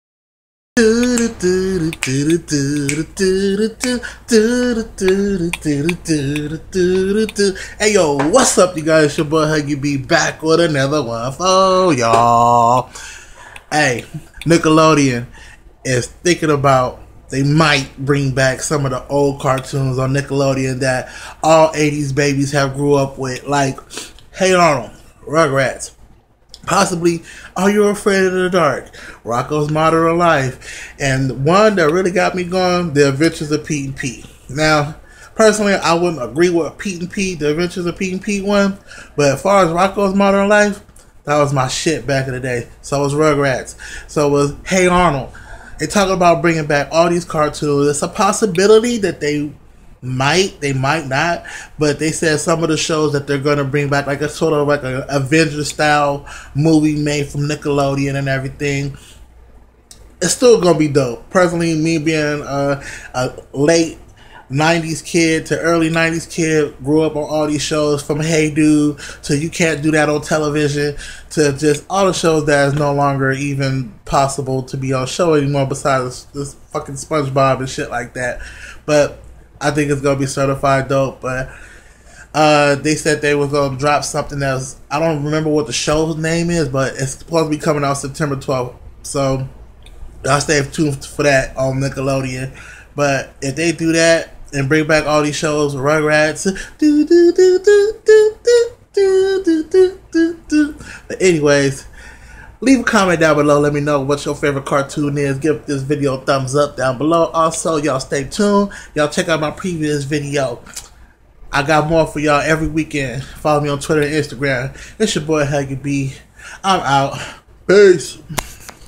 Nickelodeon may be returning classic shows
Nickelodeon announced they maybe bringing back shows as rugrats,hey Arnold ,ren and stimpy and rockos modern life if not they may make a avengers style movie and have everyone in it.thanks for watching follow me